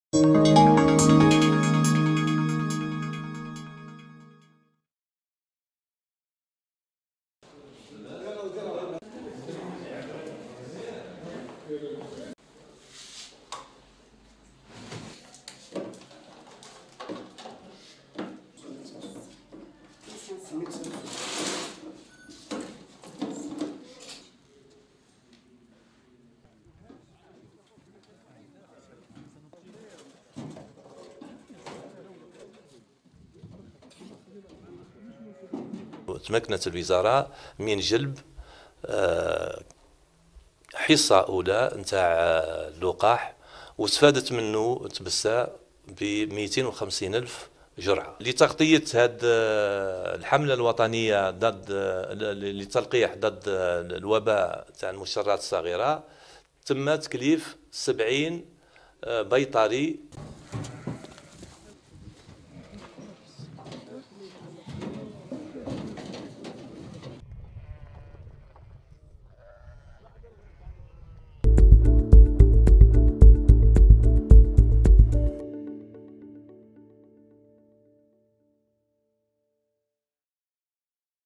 تبسة: 250 ألف لقاح ضد طاعون المجترات الصغيرة
تبسة (وأج)- أطلقت مديرية المصالح الفلاحية بتبسة حملة واسعة لتلقيح المواشي ضد طاعون المجترات الصغيرة بعد استلامها حصة أولية تضم 250 ألف جرعة لقاح، و تم  تسخير 70 طبيبا بيطريا خاصا لتأطير هذه العملية التي ستشمل المناطق التي مسها هذا الوباء.